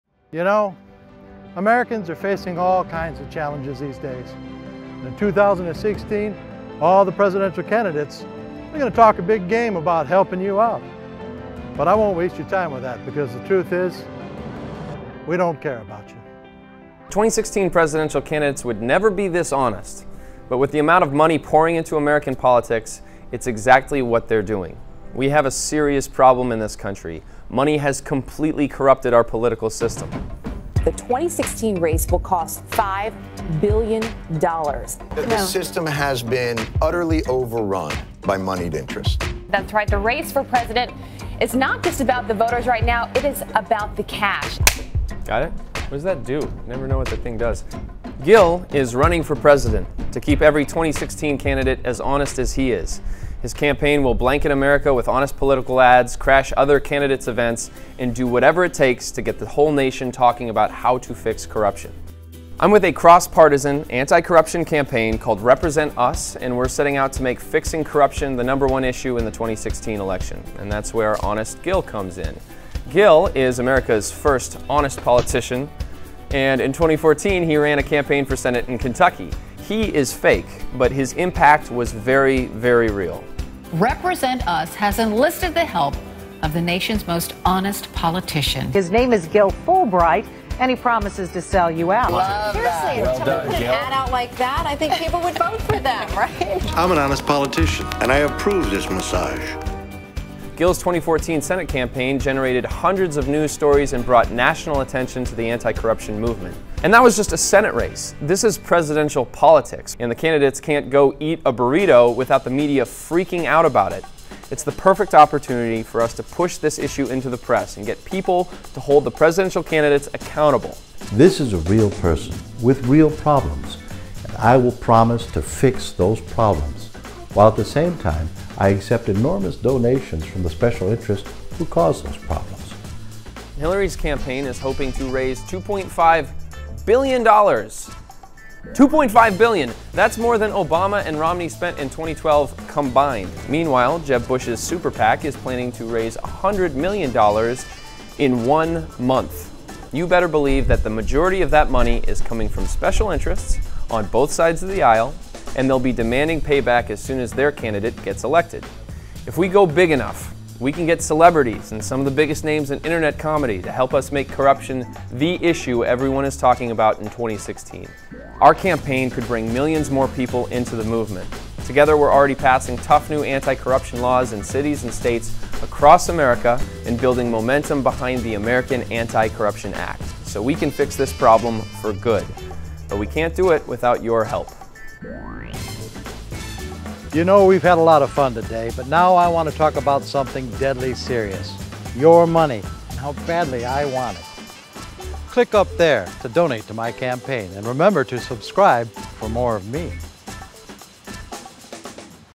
The Plan: Honest Gil for President
We’re running Gil in the most expensive presidential election in U.S. history to make sure America’s corruption crisis stays in the national spotlight for the entire 2016 campaign.